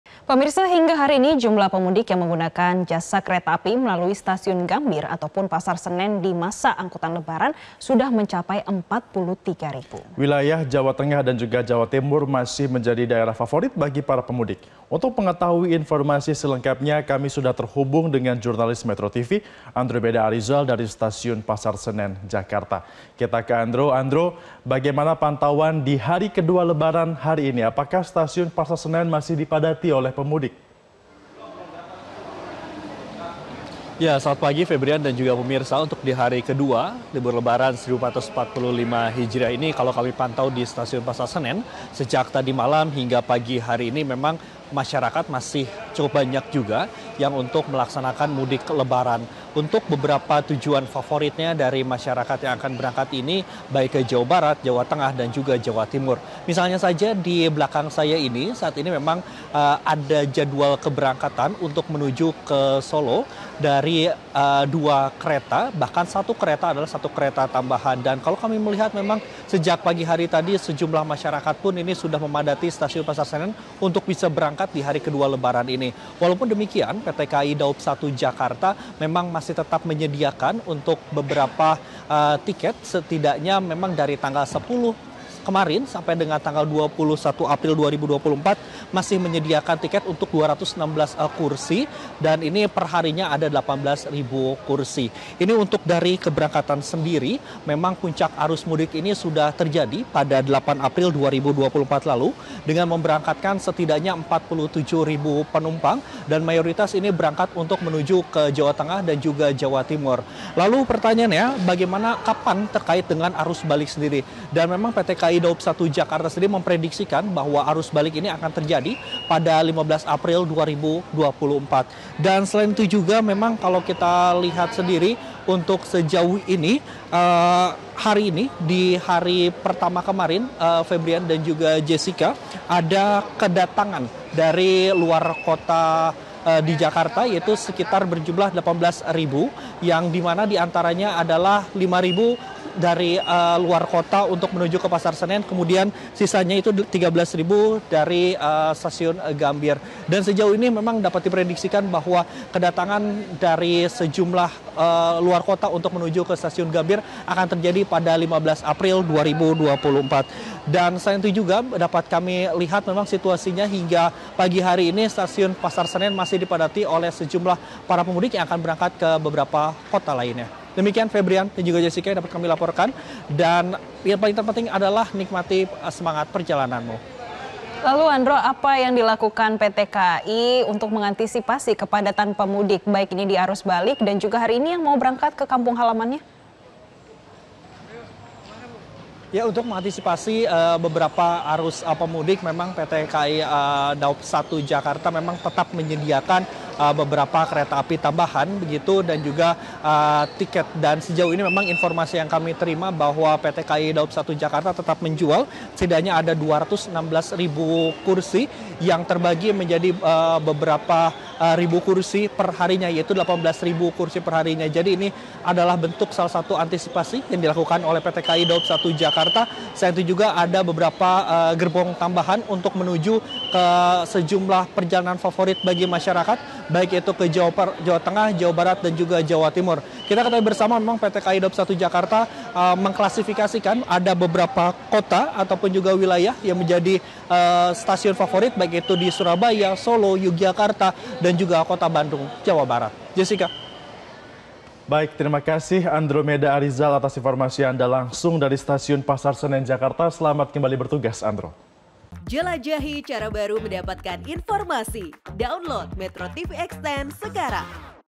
Pantauan Hari Kedua Lebaran di Stasiun Gambir & Pasar Senen
MetroTV, Jumlah pemudik yang menggunakan jasa kereta api melalui Stasiun Gambir ataupun Pasar Senen di masa angkutan lebaran sudah mencapai 43.000.